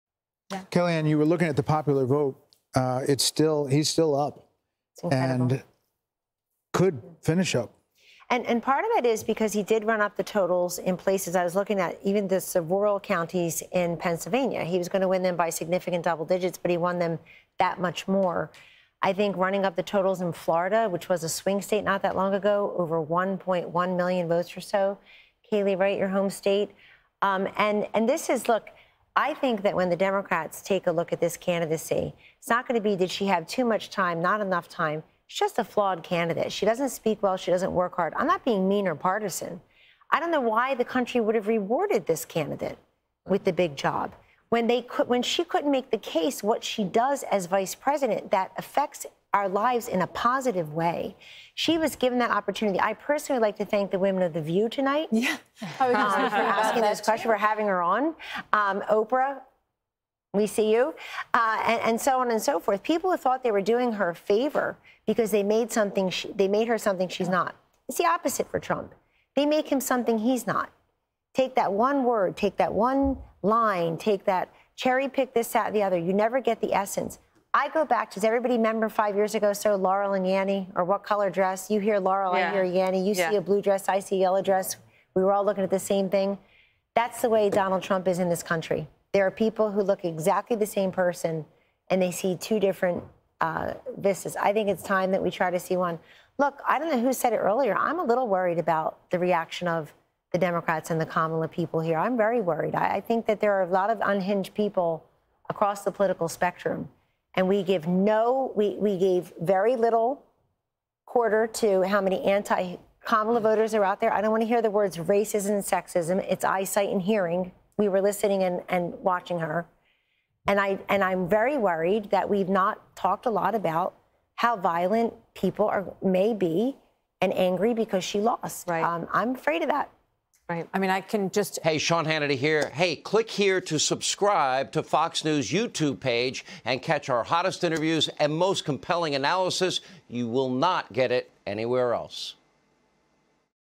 Kellyanne Conway 'personally' thanks 'The View' for Trump's victory
Fox News contributor Kellyanne Conway criticizes the way Vice President Kamala Harris ran her campaign against President-elect Trump.